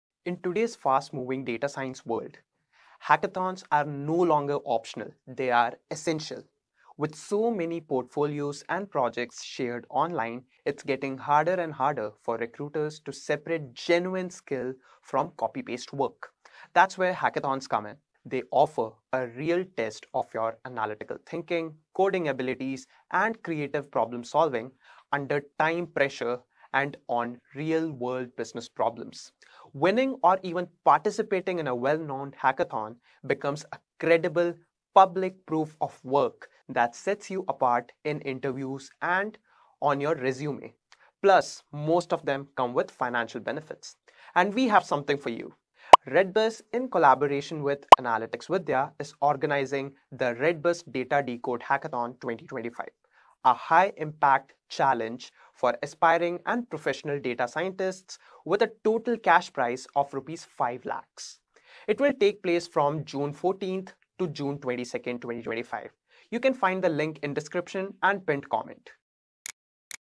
Why Should you Participate in Data Science Hackathons?
Data Scientists, listen up! 👂 Want to prove your skills and stand out? Hackathons are ESSENTIAL! Join the redBus Data Decode Hackathon 2025 in collaboration with Analytics Vidhya! Win from a ₹5 LAKHS cash prize! 🤯